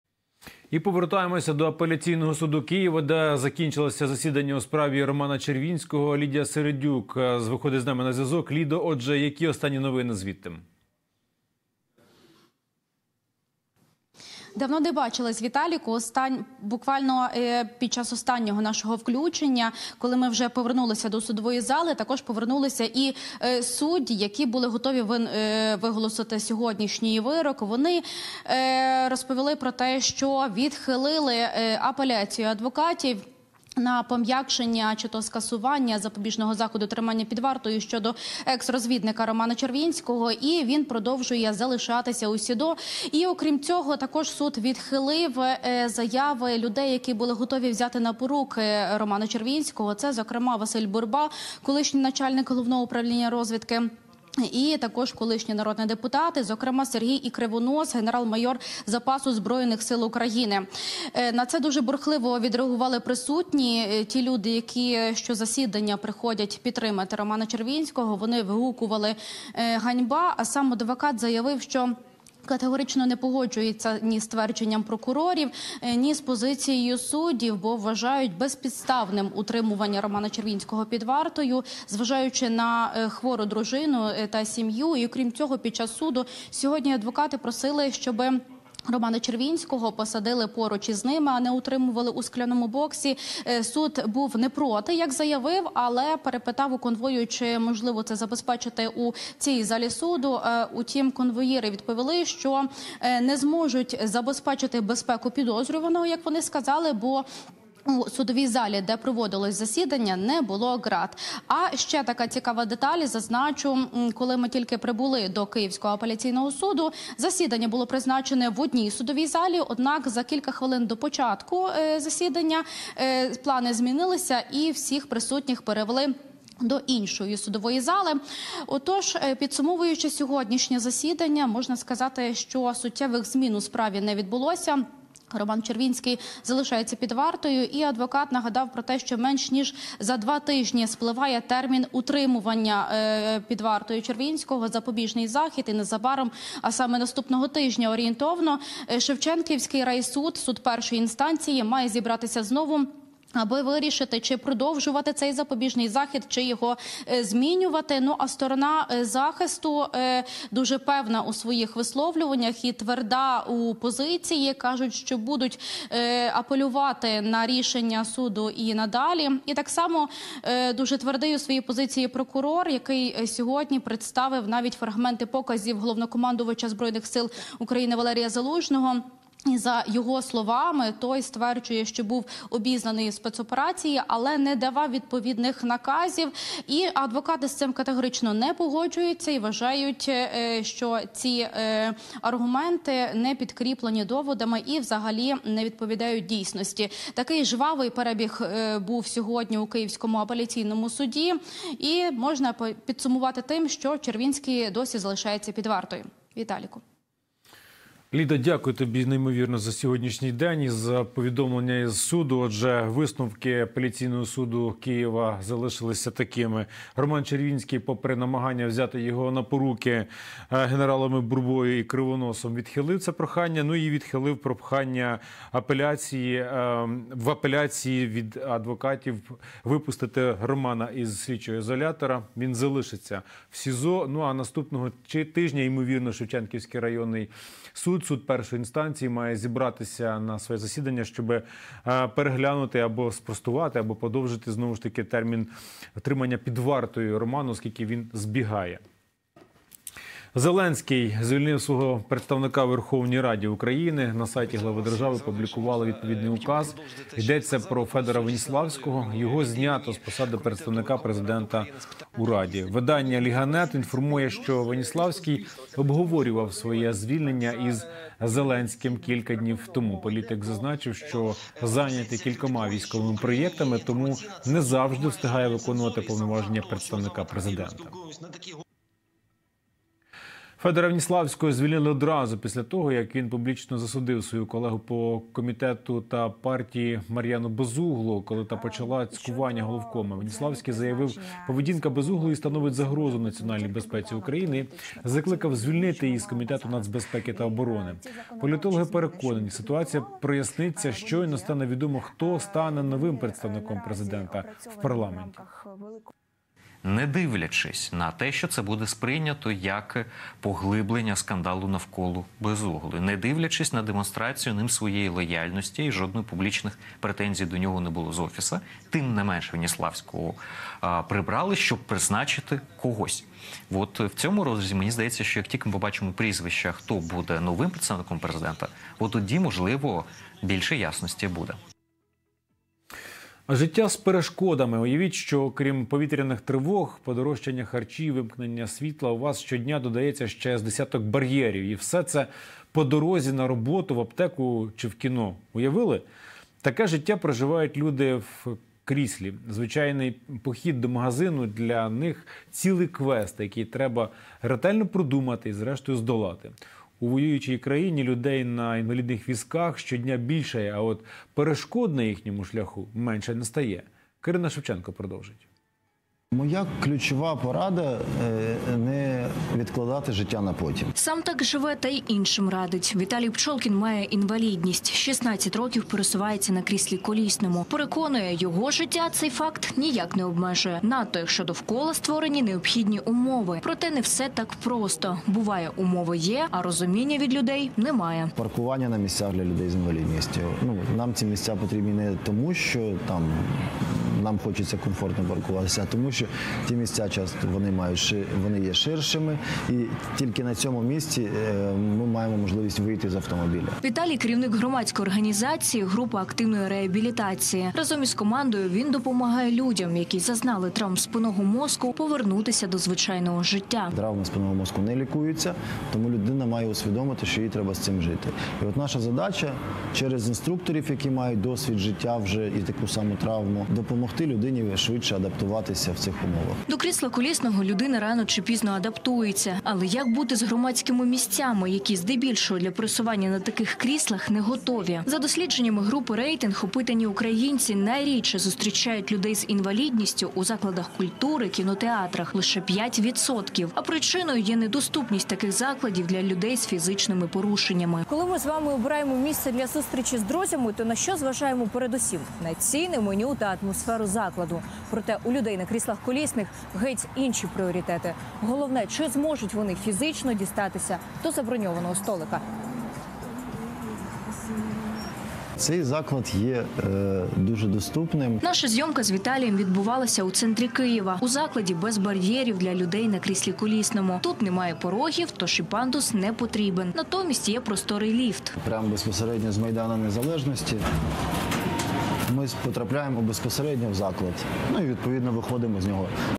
🔥ГАРЯЧІ подробиці із зали суду по справі Червінського! Яке рішення винесла Феміда?
Повертаємося до Київського апеляційного суду, де упродовж дня розглядали справу ексрозвідника Романа Червінського. Адвокати просили пом'якшити або й скасувати йому запобіжний захід. Маємо вже рішення суду. Тож який вердикт - розпитаємо у кореспондентки Лідії Середюк.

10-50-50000 - будь-яка сума має значення.

Разом переможемо!
Слава Україні!